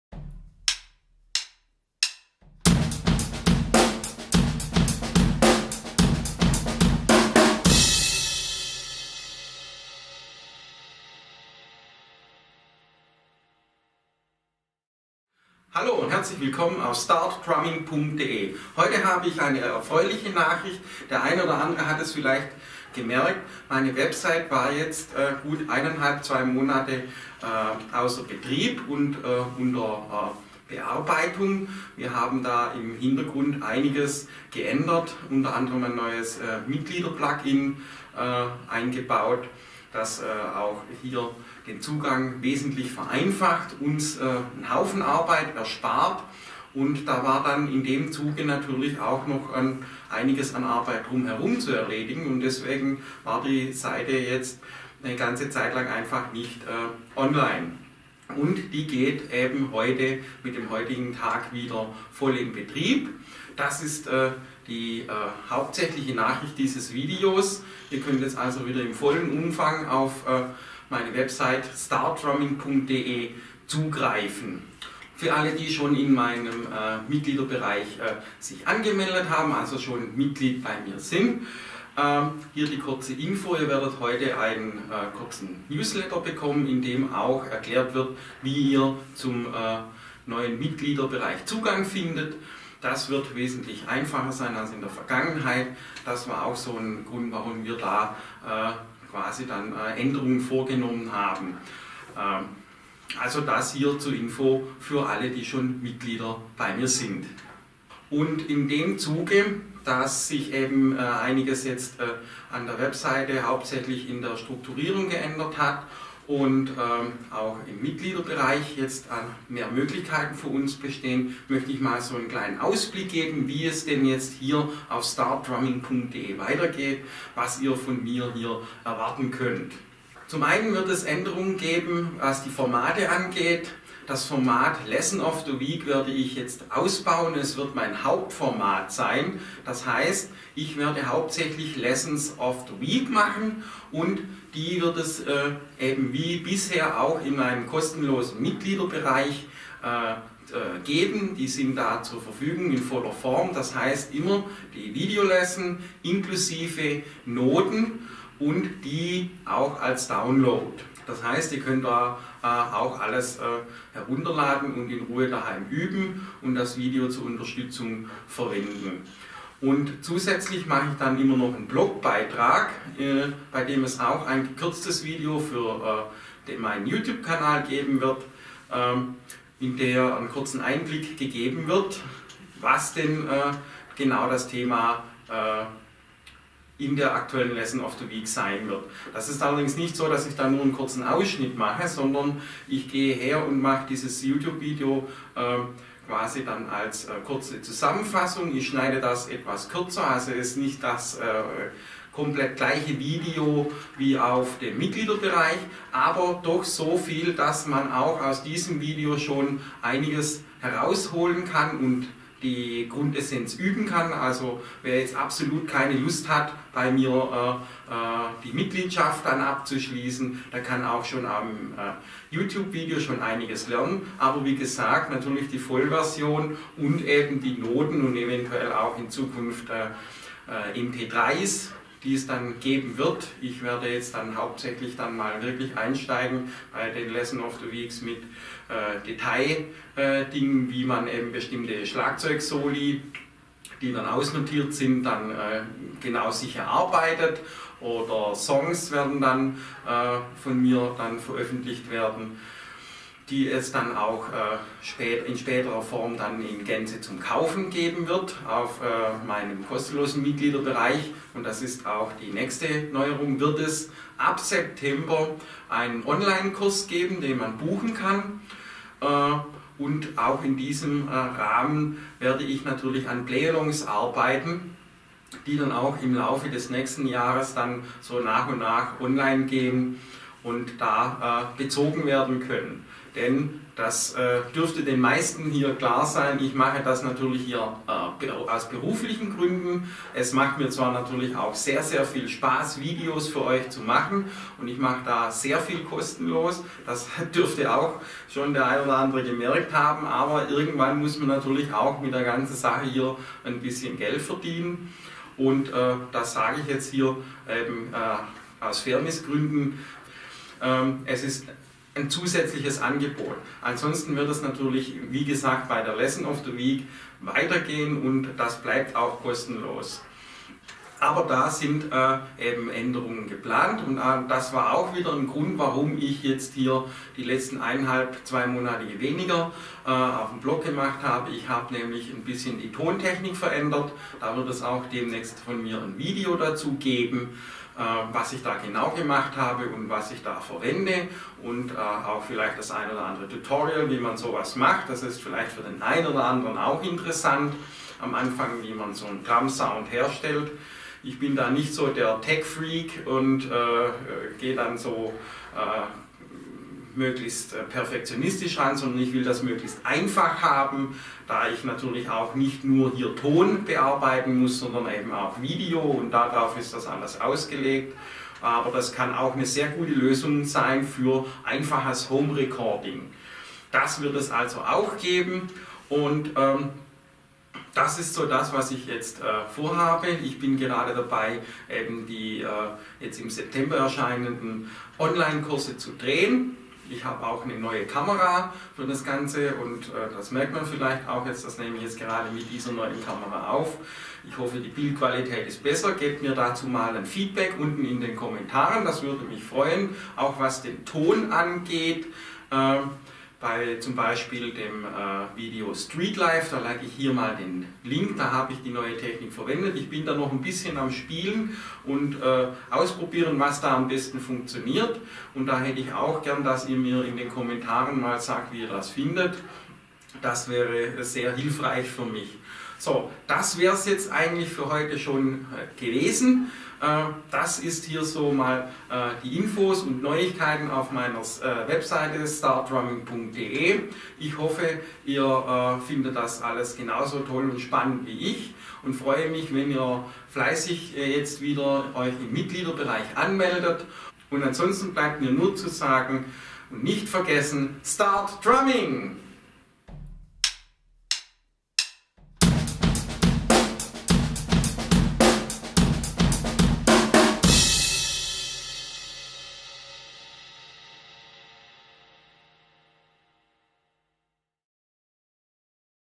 startdrumming de ist wieder Online und ein Ausblick!
Hallo zusammen,

in diesem Video habe ich ein paar kurze Infos wie es auf meiner, nun wieder erreichbaren Webseite, startdrumming.de und hier auf meinenm YT-Kanal weitergehn wird.

Warum die Webseite offline war und was isch noch alles hier vor habe erfahrt ihr hier und heute in diesem Video! :-)

Viel Spa? und nicht vergessen...

START DRUMMING! :-)